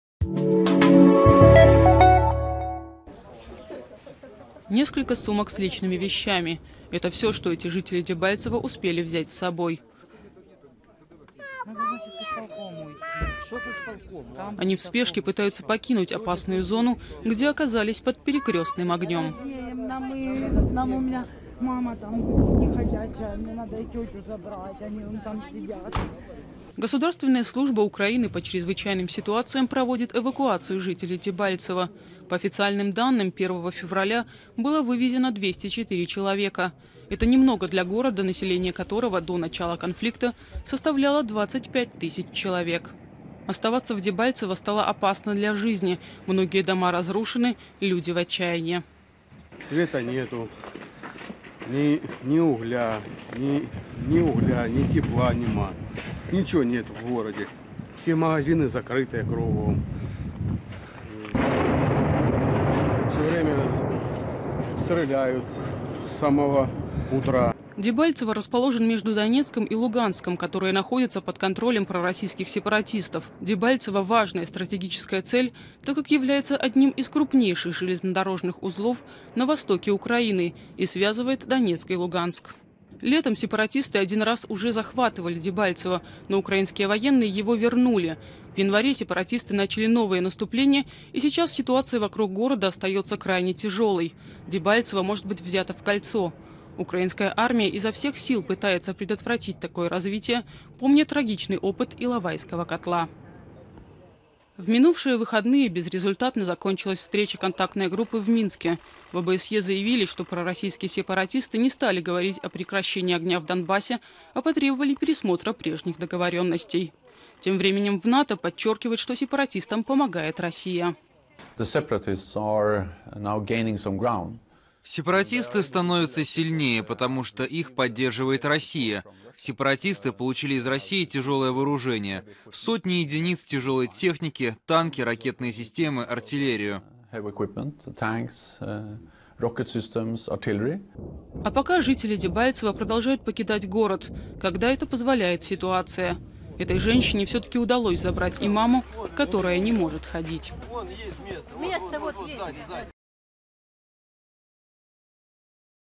Из-за сильных боев жители Дебальцево покидают город
Жители городка Дебальцево Донецкой области уезжают из города. Оставаться там опасно для жизни. Тем временем в НАТО заявляют, что Россия поддерживает сепаратистов живой силой и оружием.